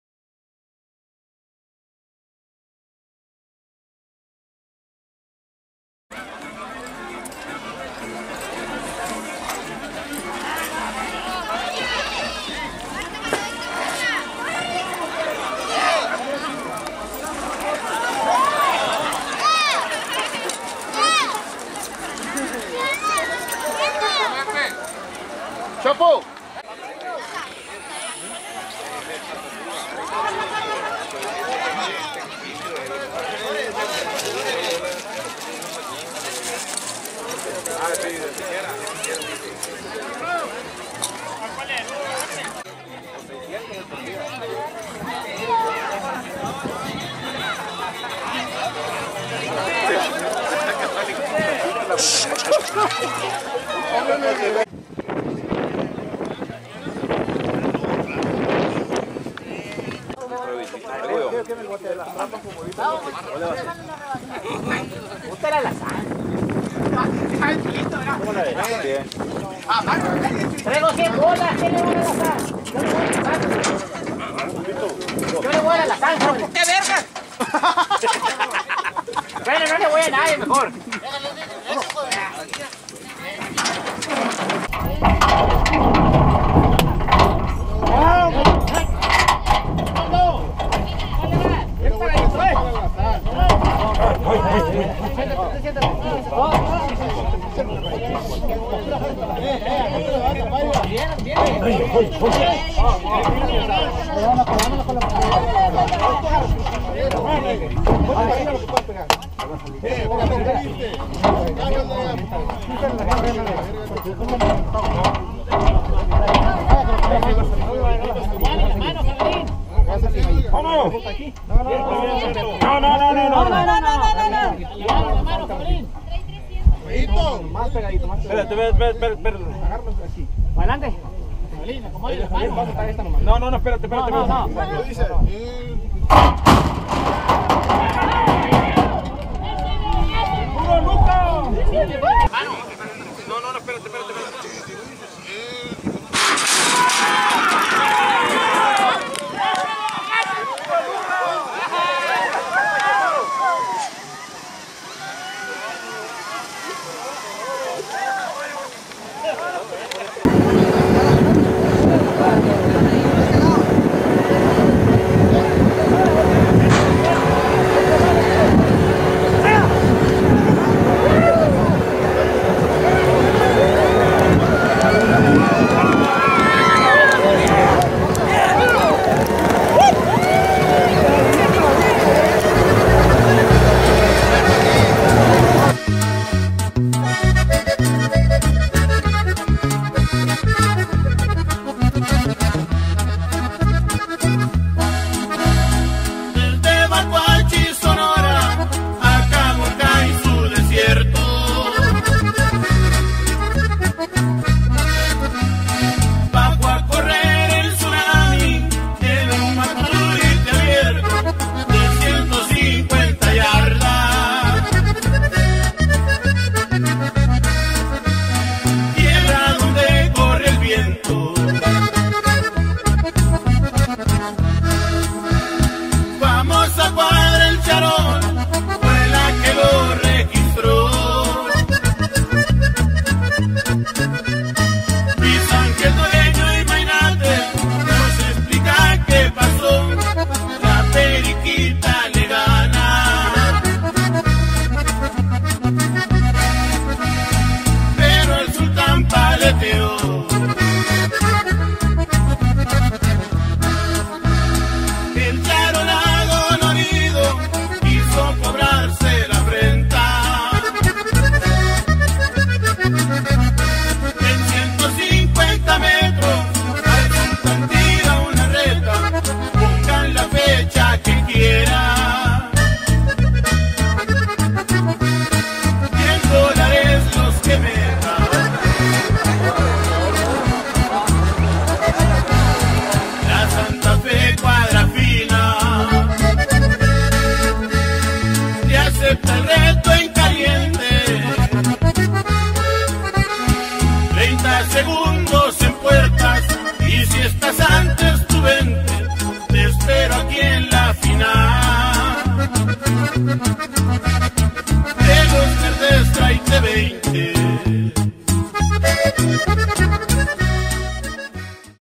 CARRERAS EN EL CLARO 21.12.31 7a sinverguenza vs chatanuga
7a carrera del viernes 31 de diciembre 2021.
Taste de Ej. "El Claro" Santa Ana, Son.